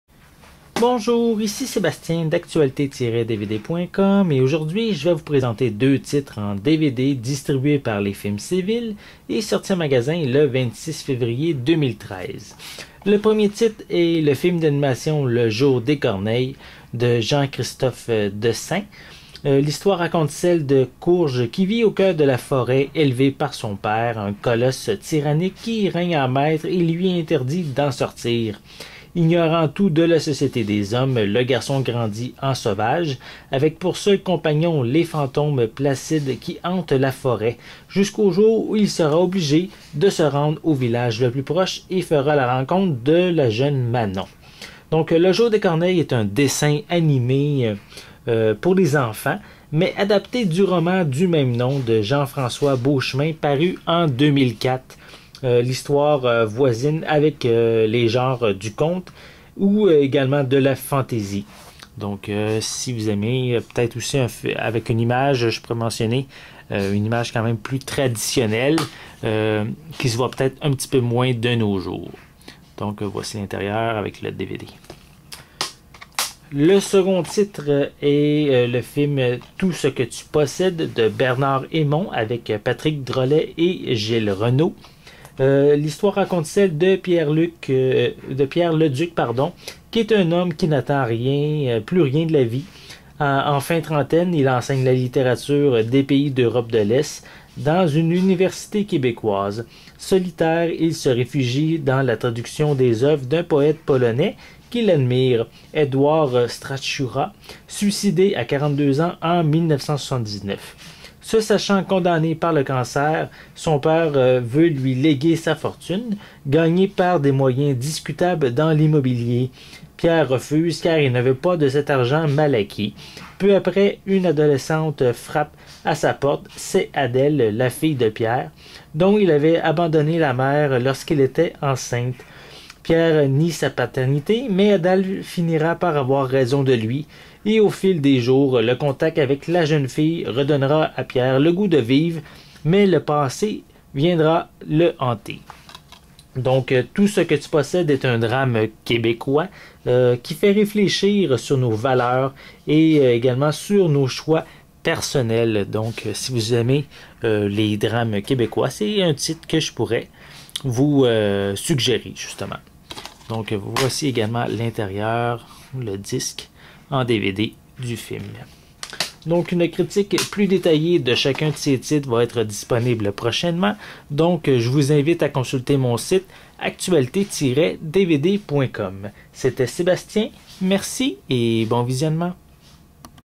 Présentation Tout ce que tu possèdes et Le jour des corneilles
Présentation Tout ce que tu possèdes et Le jour des corneilles par Sébastien pour 2 titres distribués par Les Films Séville et sortie en magasin le 26 février 2013.

Le jour des corneilles : Le fils Courge vit au cœur de la forêt, élevé par son père, un colosse tyrannique qui y règne en maître et lui interdit d'en sortir. Ignorant tout de la société des hommes, le garçon grandit en sauvage, avec pour seuls compagnons les fantômes placides qui hantent la forêt. Jusqu'au jour où il sera obligé de se rendre au village le plus proche et fera la rencontre de la jeune Manon.

Tout ce que tu possèdes : Pierre Leduc est un homme qui n'attend plus rien de la vie. En fin de trentaine, il enseigne la littérature des pays d'Europe de l'Est dans une université québécoise. Solitaire, il se réfugie dans la traduction des œuvres d'un poète polonais qu'il admire, Edward Stachura, suicidé à 42 ans en 1979. Se sachant condamné par le cancer, son père veut lui léguer sa fortune, gagnée par des moyens discutables dans l'immobilier. Pierre refuse, car il ne veut pas de cet argent mal acquis. Peu après, une adolescente frappe à sa porte : c'est Adèle, la fille de Pierre, dont il avait abandonné la mère lorsqu'elle était enceinte. Pierre nie sa paternité, mais Adèle finira par avoir raison de lui. Au fil des jours, le contact avec la jeune fille redonnera à Pierre le goût de vivre. Mais le passé reviendra le hanter...